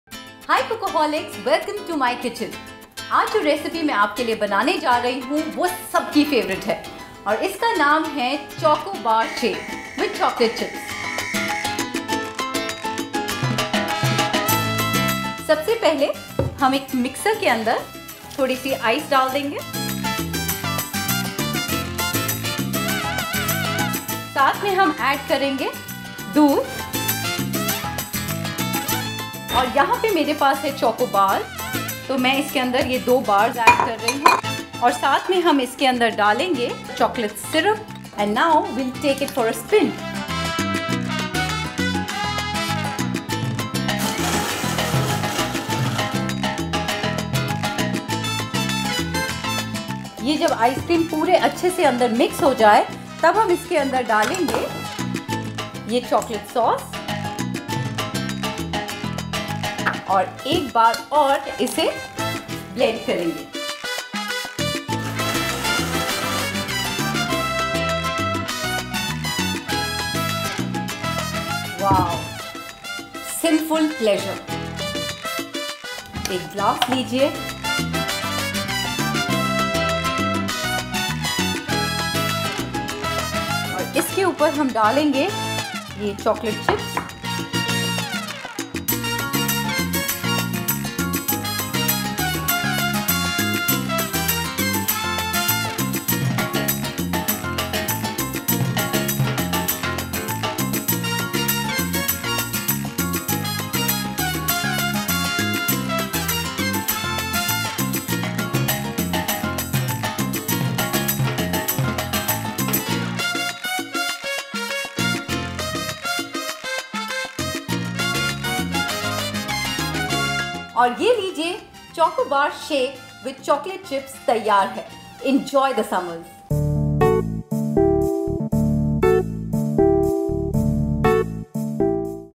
Chocobar shake with Chocolate Chips
Learn to get some sinful pleasure recipe of choco bar shake with Chef Shipra Khanna